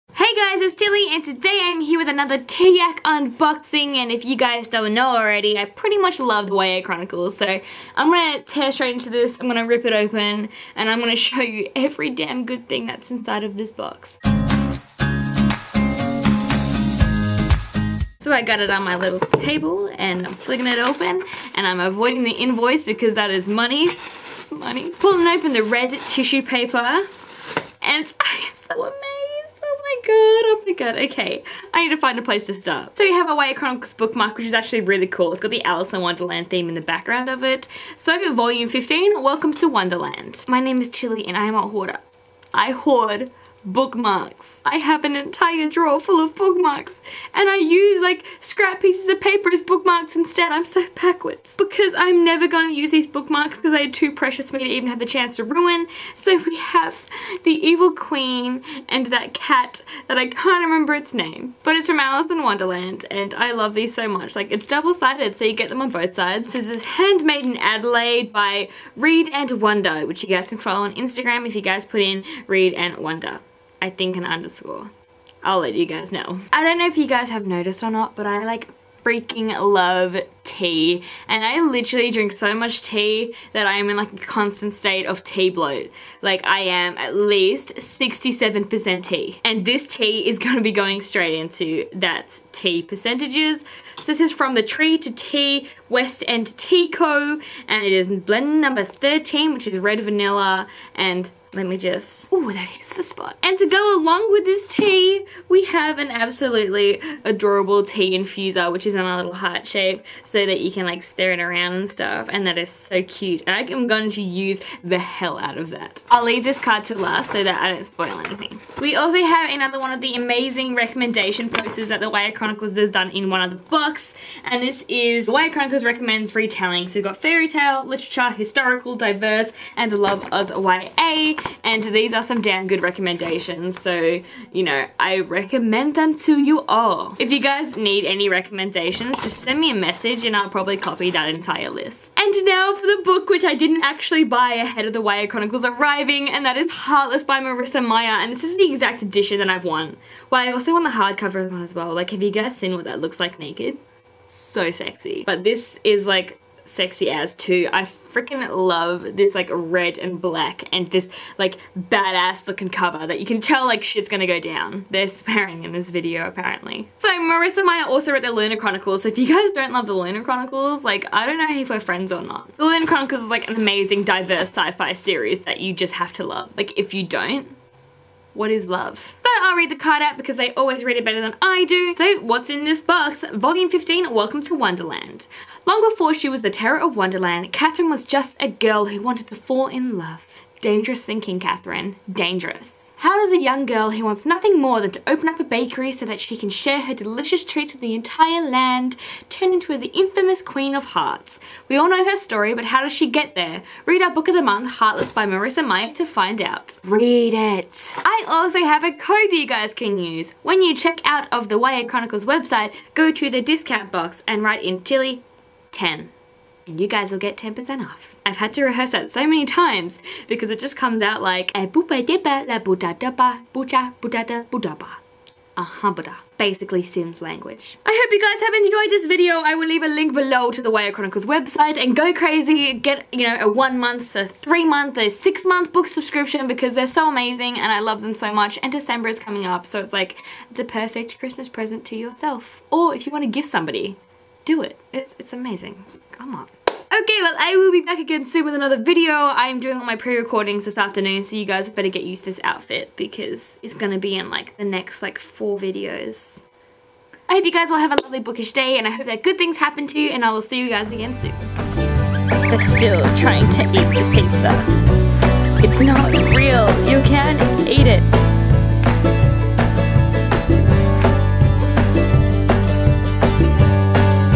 TYAC UNBOXING: WELCOME TO WONDERLAND
The YA Chronciles still had this wonderful Novmeber box available so you've still got time to get your hands on it!